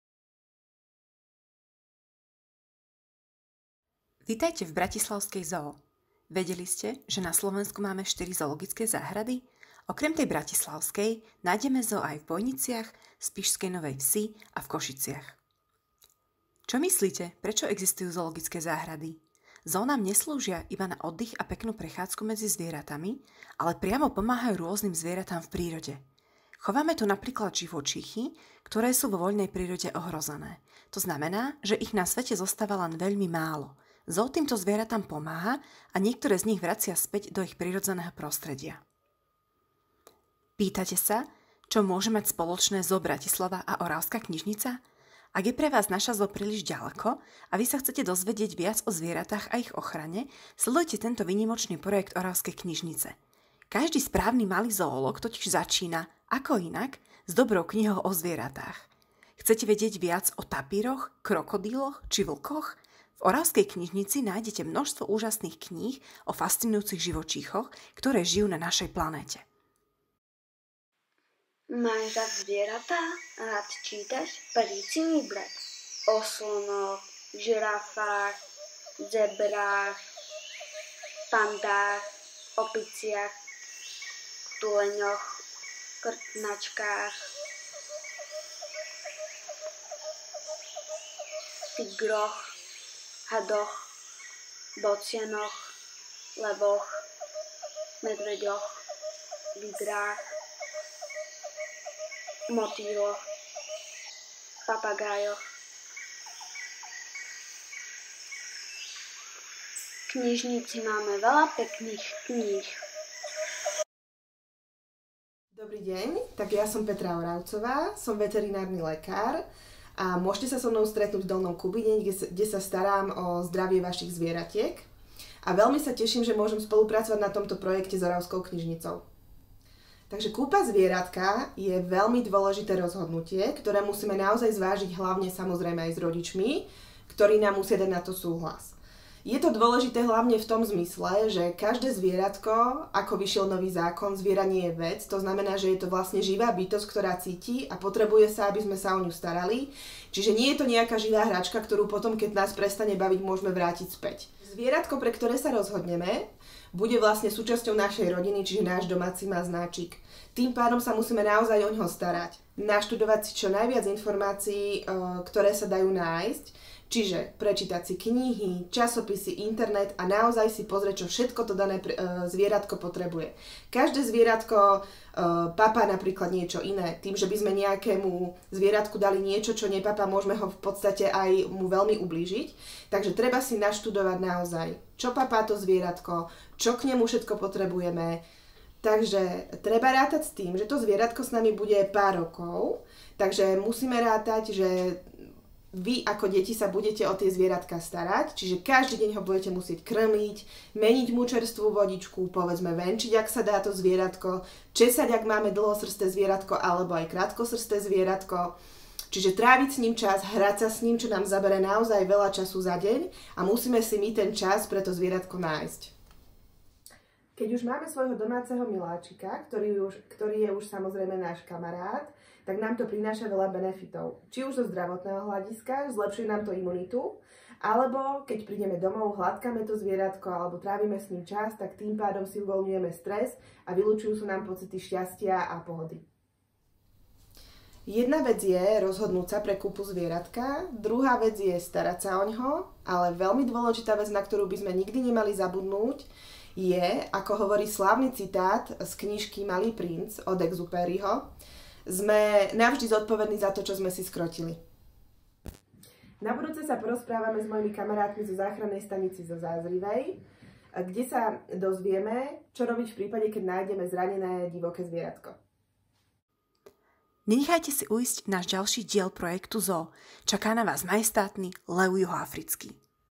Do ZOO a do knižnice
A je tu prvá časť nášho nového projektu "Za zvieratami do ZOO a do knižnice", v ktorej sa spolu s nami vyberiete na virtuálnu prehliadku do ZOO a nazrieme aj do kníh o zvieratkách.
S novou rubrikou nezačíname náhodou dnes, 30. apríla. Dnes je totiž Svetový veterinárny deň, ktorý je oslavou veterinárnej profesie. Pripomína sa od roku 2001 a bol vyhlásený Svetovou veterinárnou asociáciou. Zámerom je zvýšiť povedomie o väzbách medzi zvieratami a zdravím ľudí, pretože mnohé ľudské choroby majú pôvod v živočíšnej ríši.
A preto v našom novom projekte nebudú chýbať ani zaujímavé rady a tipy k starostlivosti o zvieratká, ktoré prezradí pani veterinárka. No a skvelé knihy z relácie nájdete aj na detskom oddelení na špeciálnej VÝSTAVKE ZOO A KNIŽNICA - príďte si ich požičať :)
A nezabudnite, aj zvieratkám už chýbate. Ak môžete, navštívte ich alebo ZOO Bratislava aj inak podporte. INAK - už majú tiež otvorené :)

Projekt pre vás pripravuje Oravská knižnica Antona Habovštiaka, ZOO Bratislava, Záchranná stanica pre zranené živočíchy v Zázrivej a MVDr. Petra Oravcová.